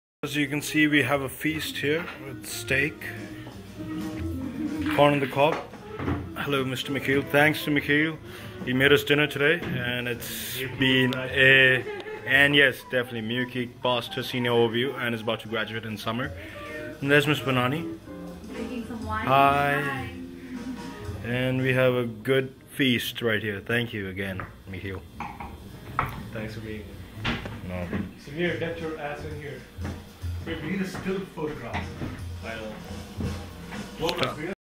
Michiels cooking dinner for us
Well before Michiel left back for Netherlands, he made some steaks which turned out to be awesome